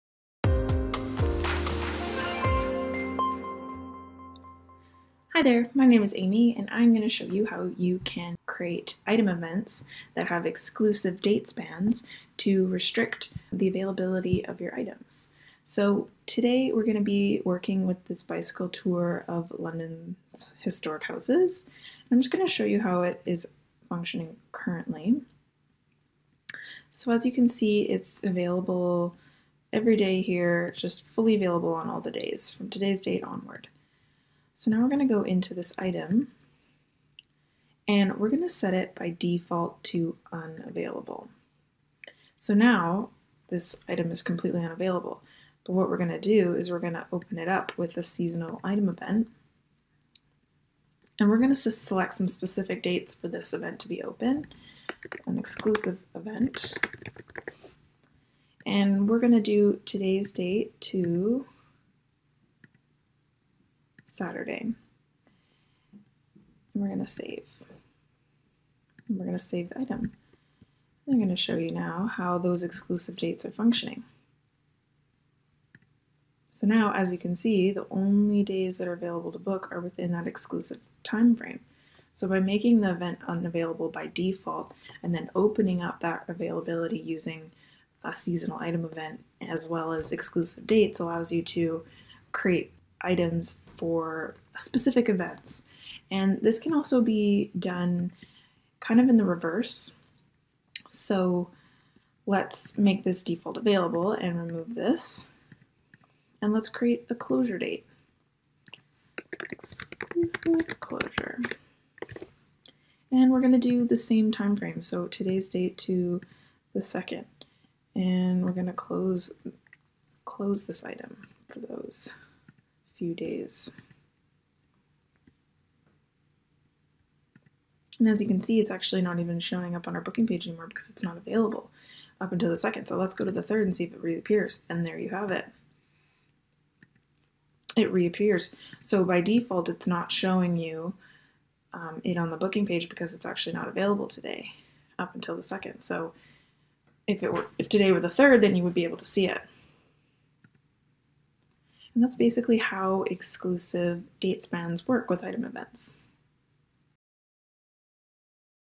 Setting Up Exclusive Availability Dates Of Items
Amy walks us through setting up exclusive availability for items including both opening and closing for specific dates.